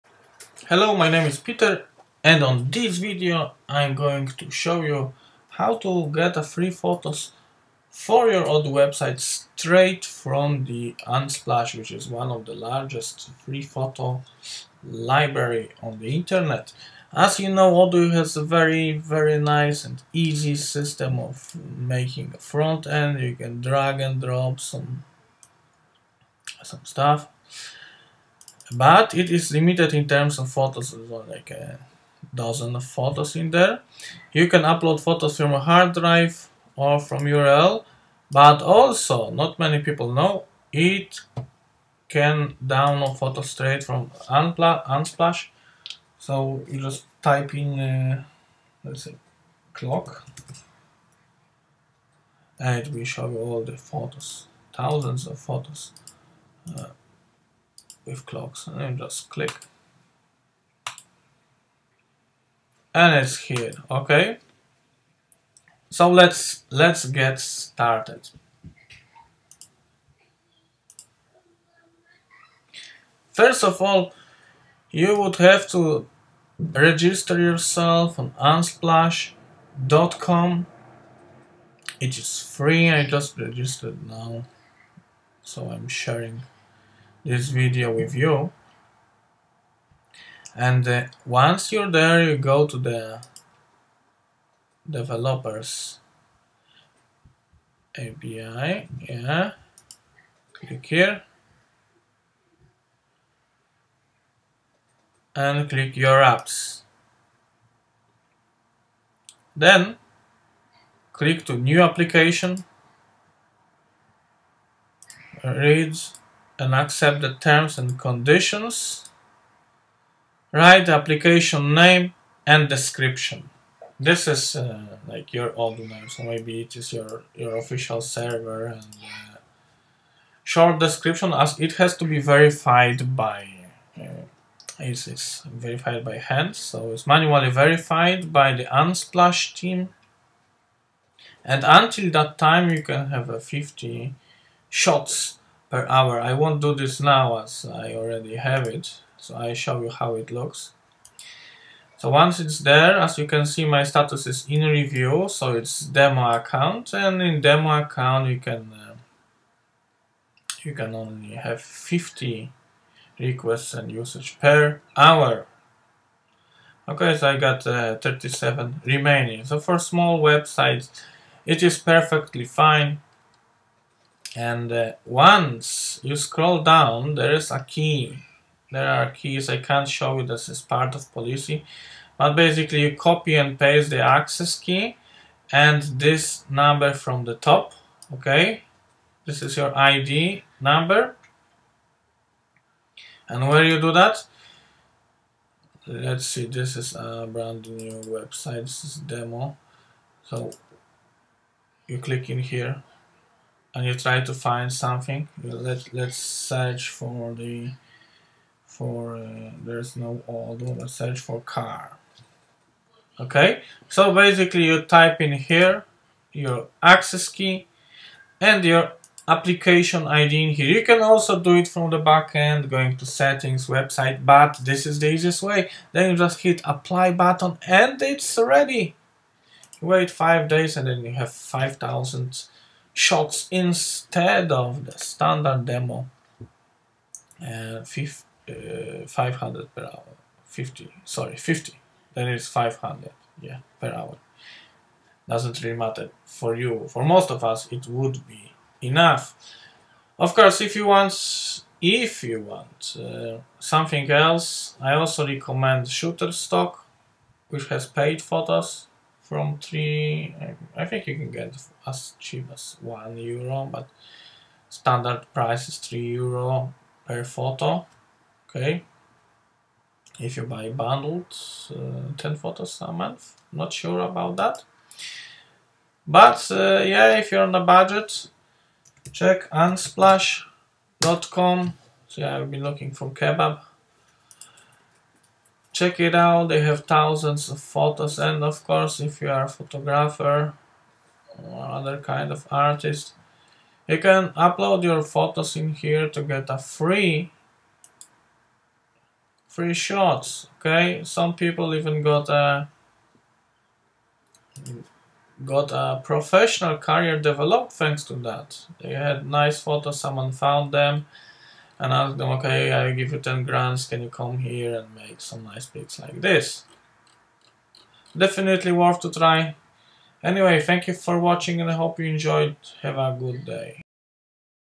How to get free photos from Unsplash in your Odoo
How to get free photos from Unsplash in your Odoo. It is possible thanks to built-in integration Odoo - Unsplash.